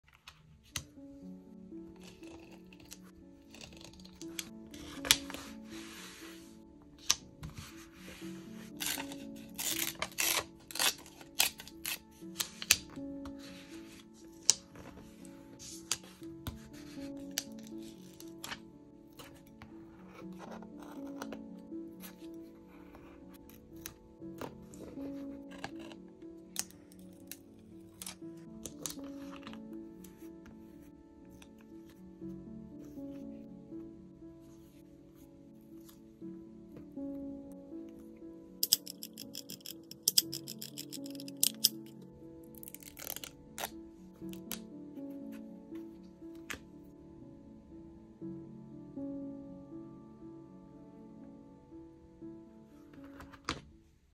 ASMR Journaling Short | Green Collab Front Cover | The Juliet Journal
ASMR journaling, with music. Decorate my ‘collab’ journal front cover with me. I will use this book for my TikTok collabs with other creative / junk journalists.

This is my only account.